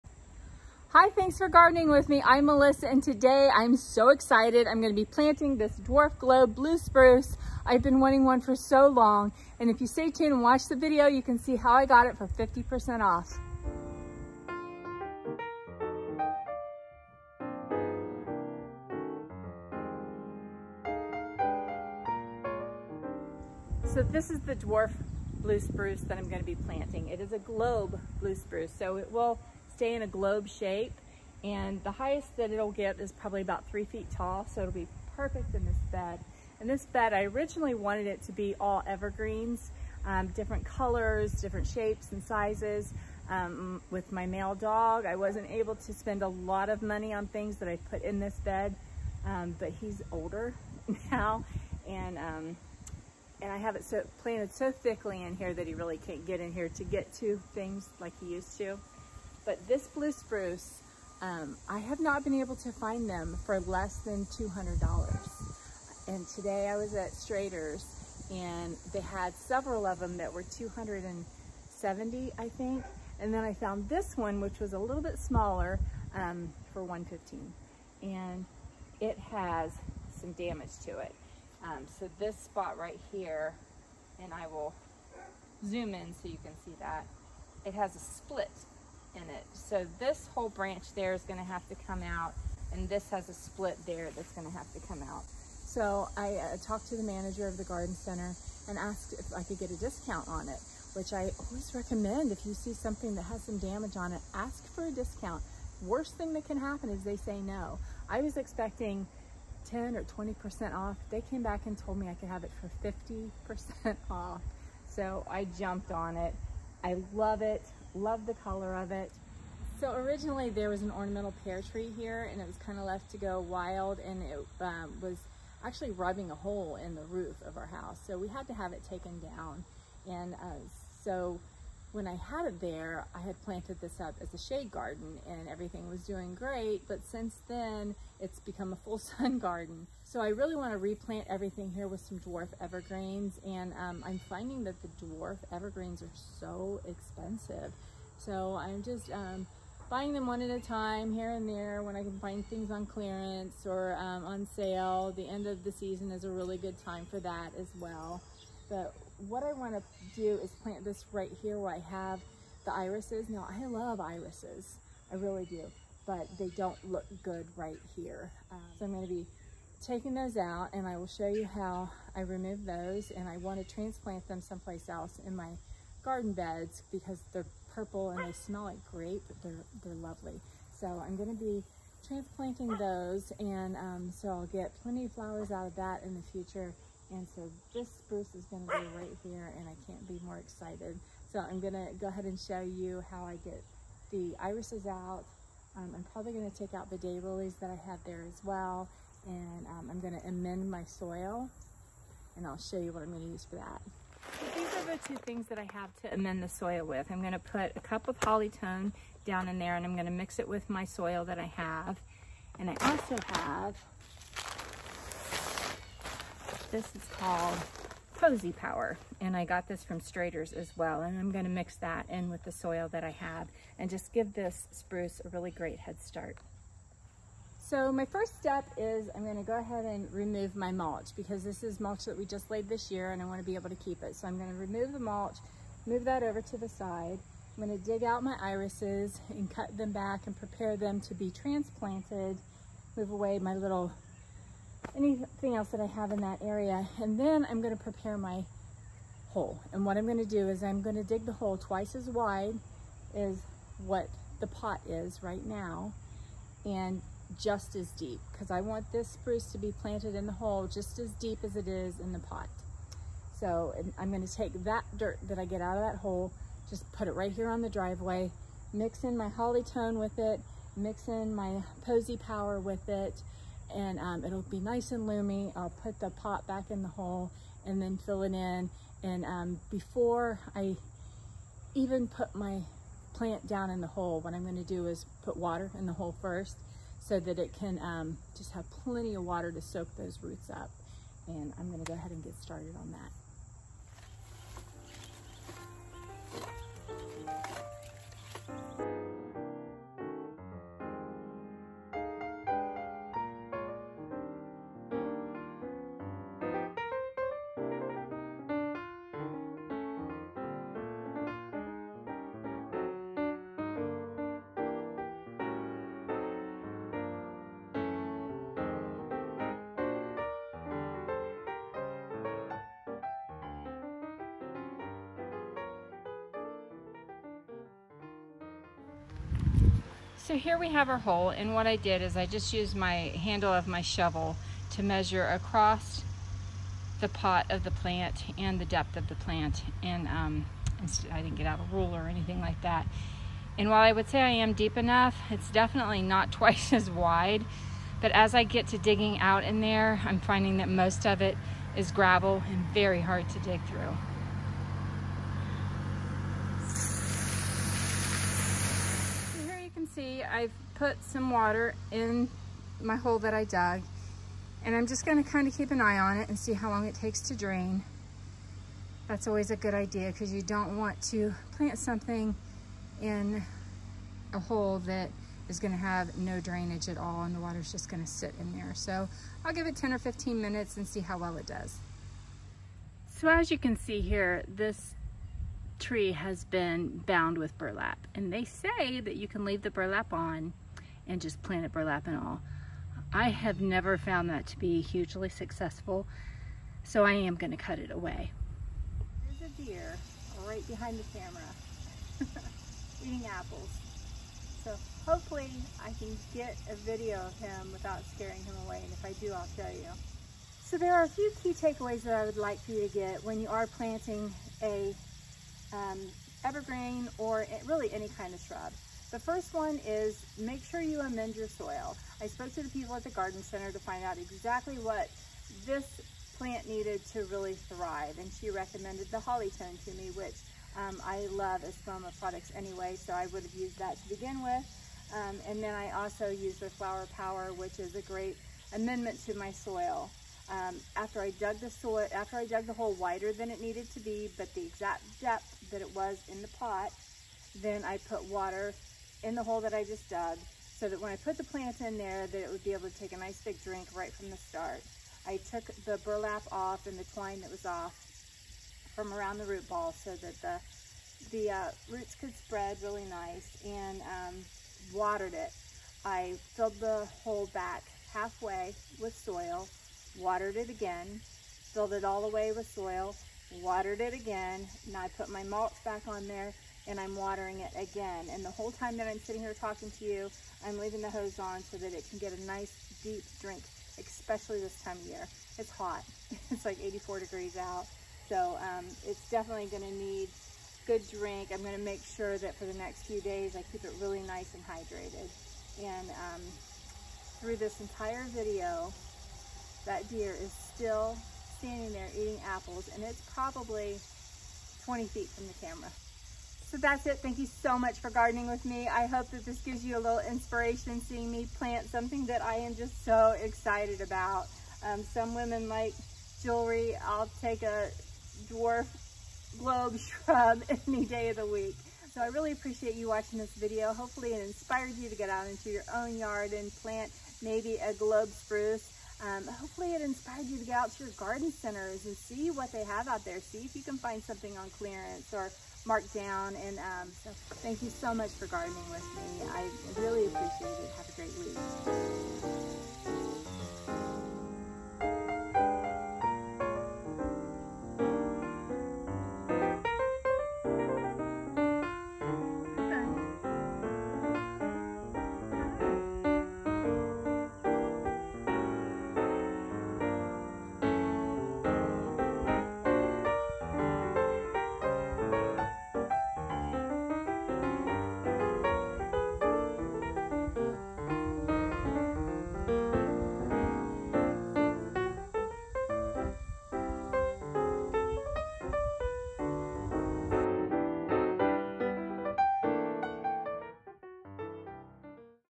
Dwarf Globe Blue Spruce -- How I got this for 50%
This video is about Dwarf Globe Blue Spruce -- How I got this for 50% and how to plant